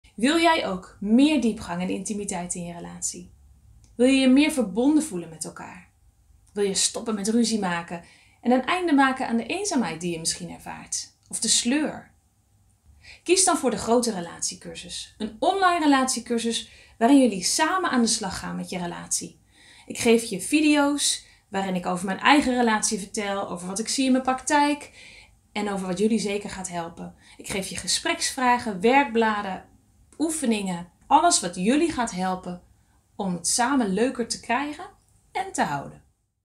Meer diepgang en intimiteit in je relatie
Zo krijg je het samen weer leuk!

Ga intensief aan de slag met je relatie, om er (weer) een fijne, sprankelende, veilige en levendige relatie van te maken waarin je allebei tot je recht komt.

Wil je meer diepgang, verbondenheid en intimiteit in je relatie? Een einde maken aan ruzies, eenzaamheid of sleur?

Met deze online relatiecursus gaan jullie samen aan de slag met je relatie. In meer dan 25 video's deel ik met je wat je moet weten over je relatie, zowel vanuit mijn vak als relatietherapeut als vanuit mijn persoonlijke ervaring.

De online relatiecursus bevat video's, gespreksvragen, oefeningen, werkbladen, tips en links.

Dit gaat jullie helpen om het samen leuker te krijgen én te houden!